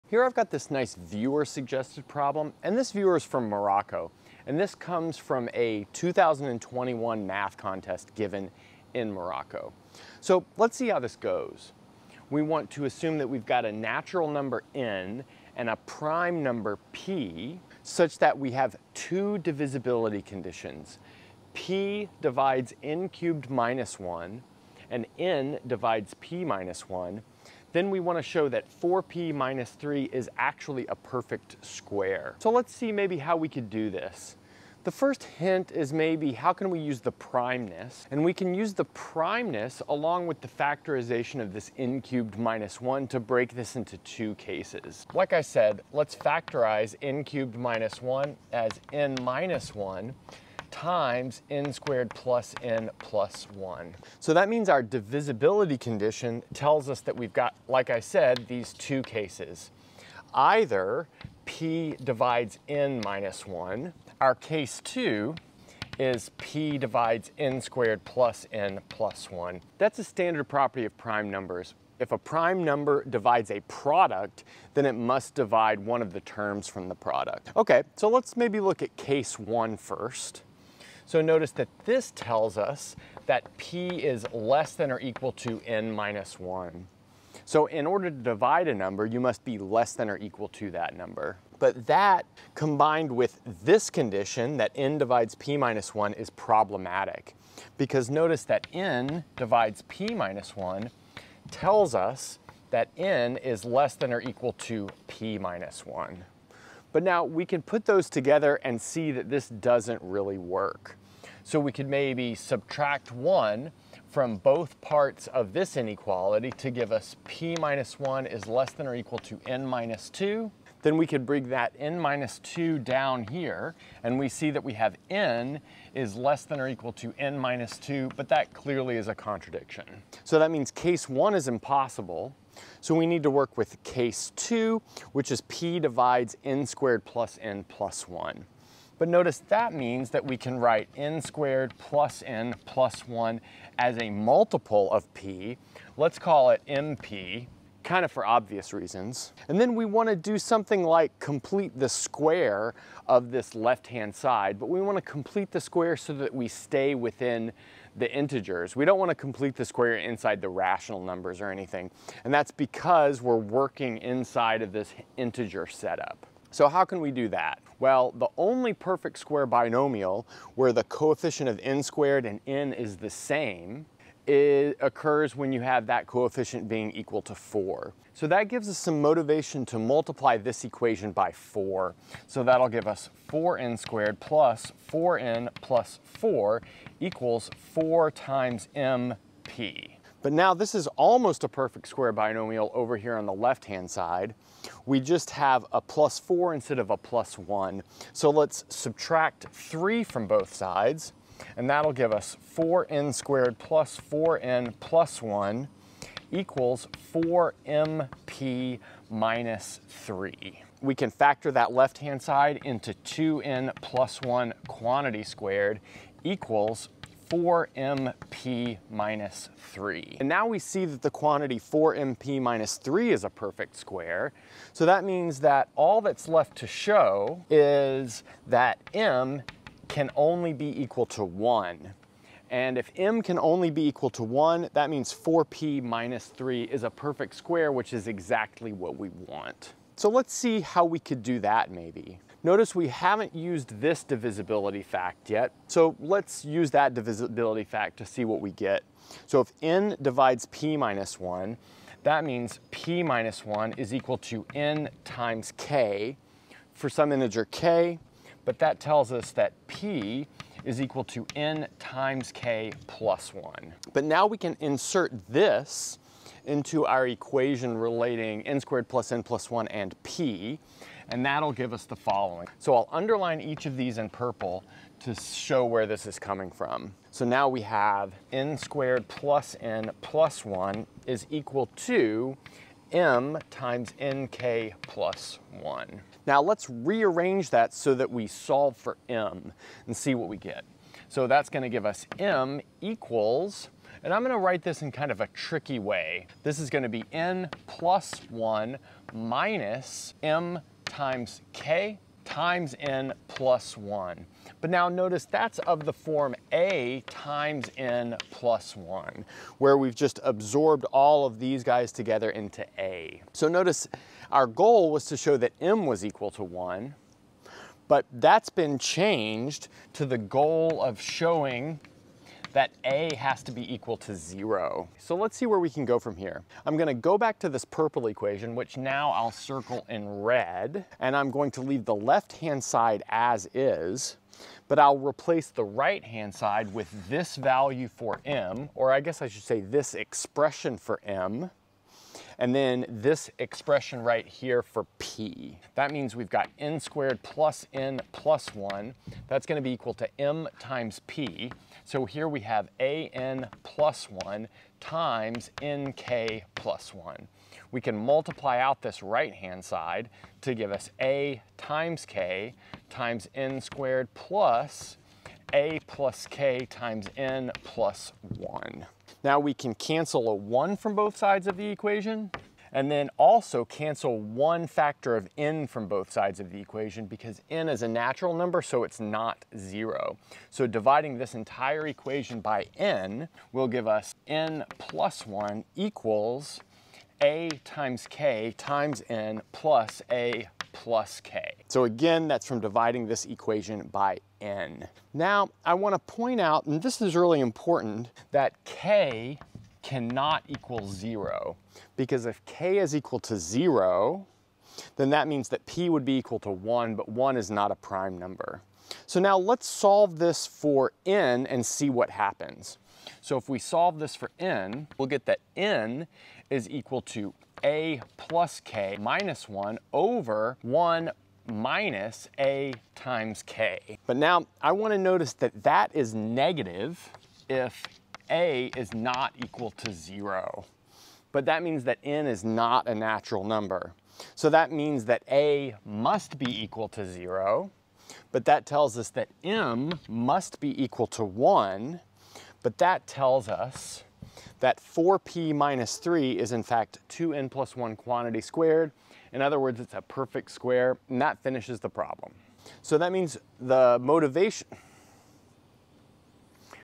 Thanks to the Moroccan viewers!
We look at a nice viewer suggested problem from Morocco.

Differential Forms: